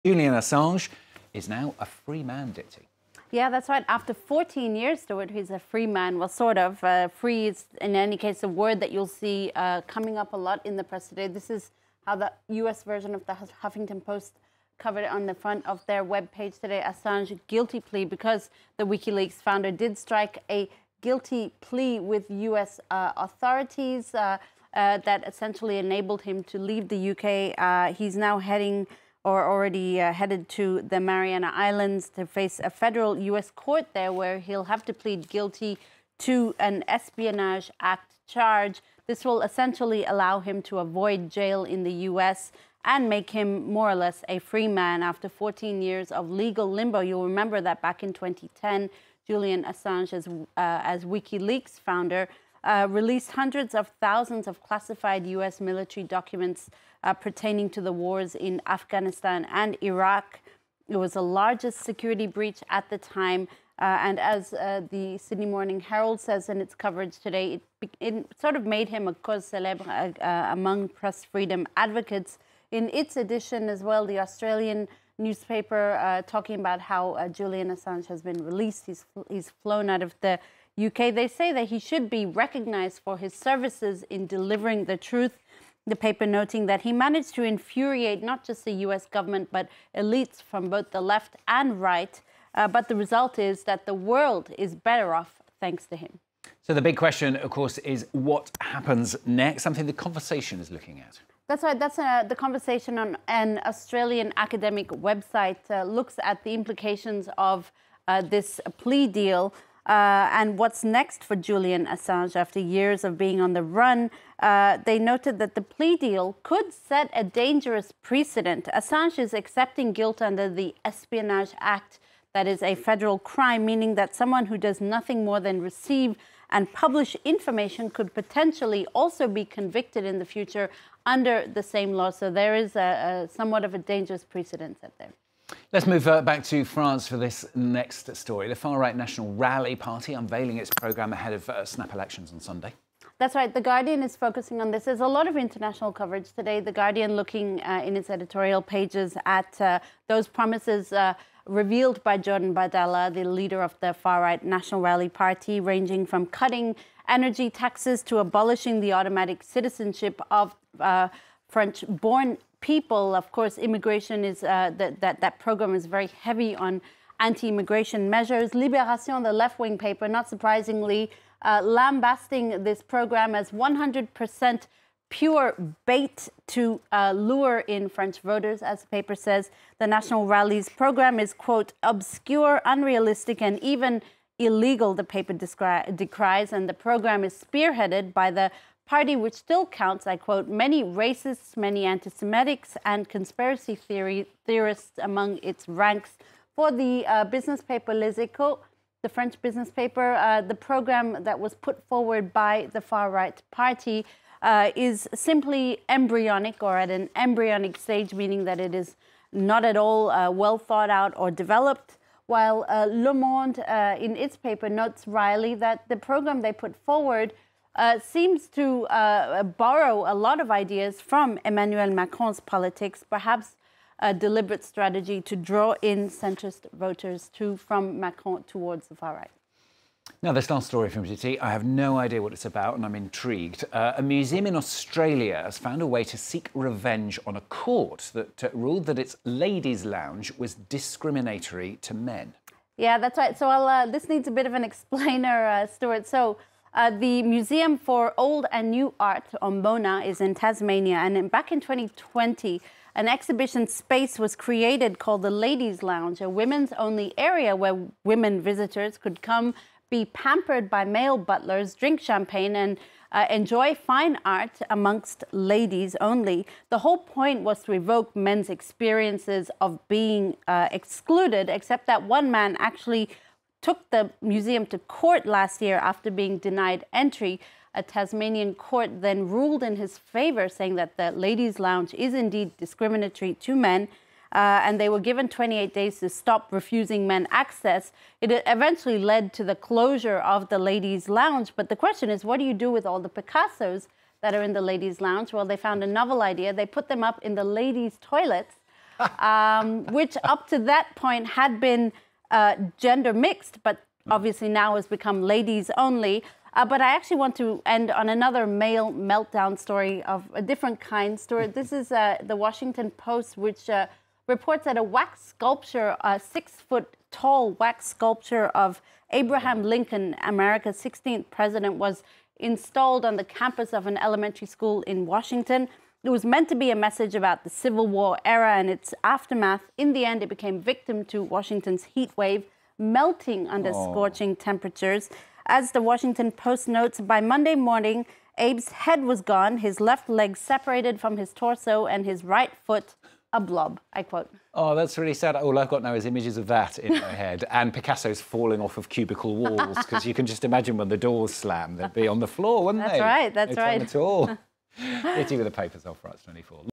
WikiLeaks founder Julian Assange finally free after 14 years of legal limbo • FRANCE 24 English
PRESS REVIEW – Tuesday, June 25: We look at reactions in the Australian press after WikiLeaks founder Julian Assange strikes a plea deal that ends 14 years of legal limbo over his publication of classified military documents. He will arrive home in Australia as a free man. Also: French papers lambast the far-right National Rally's political programme, which one says is merely "embryonic". Finally, an Australian museum finds the perfect revenge to a court ruling accusing it of discriminating against male visitors!